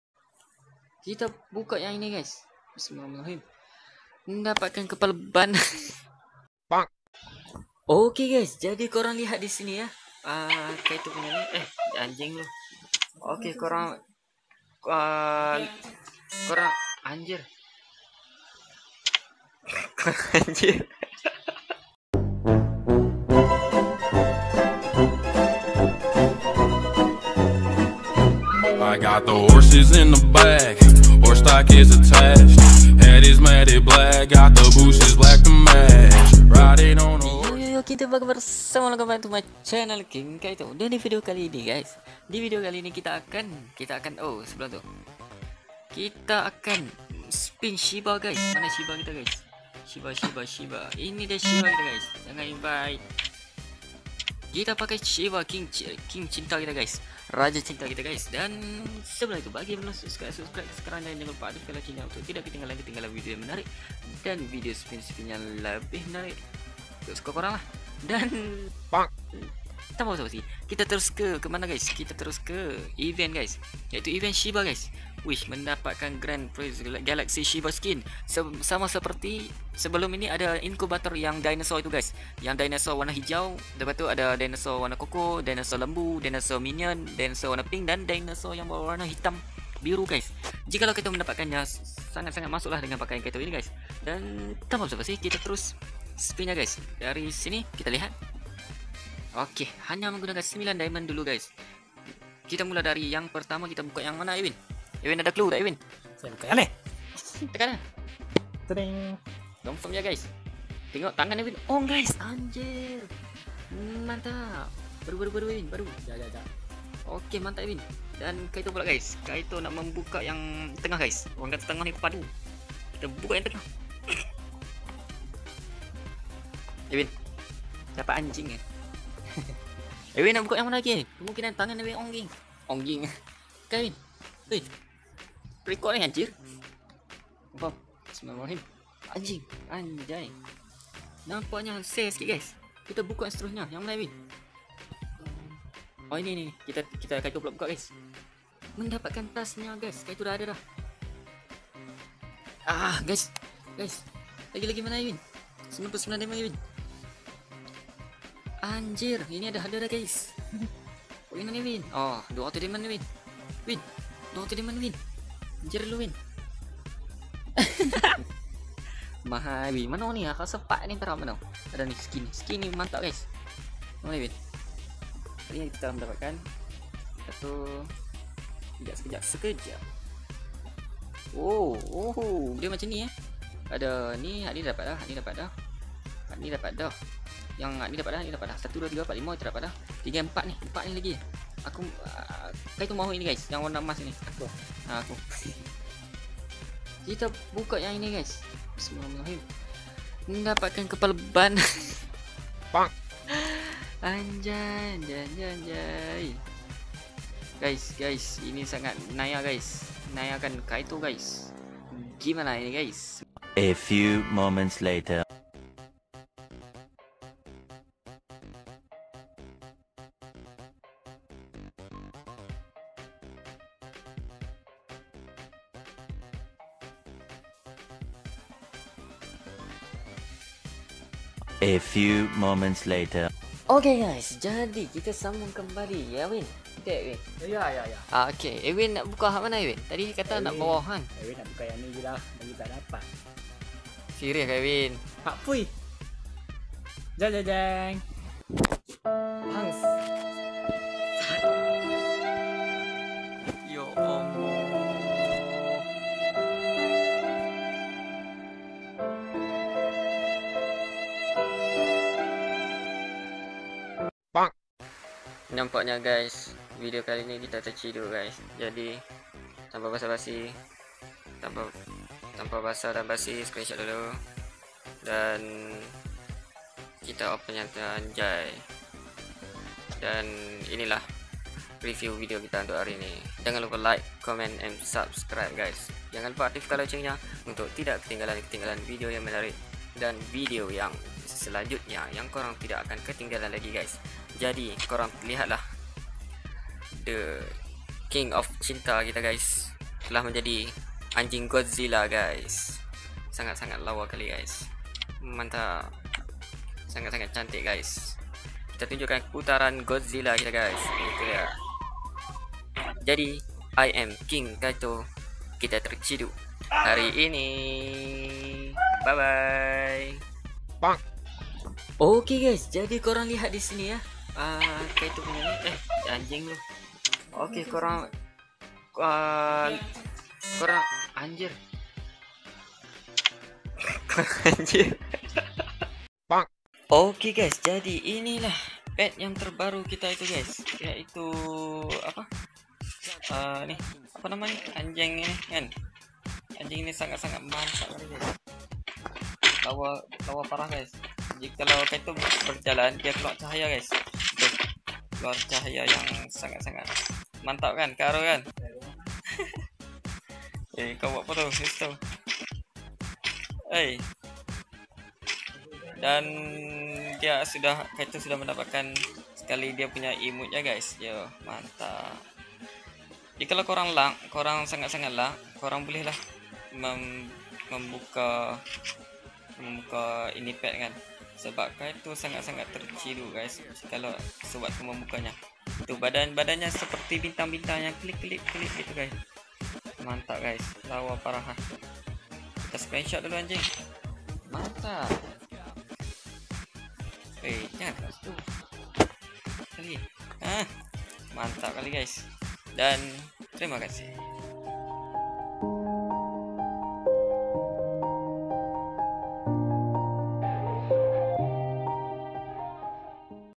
EVENT SPIN (SHIBA STRIKE) PALING TERCIDUKK GUA GUYS! DEKAT 2000RIBU DIAMOND DI BAKAR! TERUSKOYAK!
DAN DI VIDEO KALI INI KITA SPIN SHIBA STRIKE PALING TERCIDUKK GUA GUYS DAN SEMOGA KORANG TERHIBUR DGN VIDEO INI GUYS DAN TERIMA KASIH...
SEMOGA TERHIBUR DAN MENGHIBURKAN!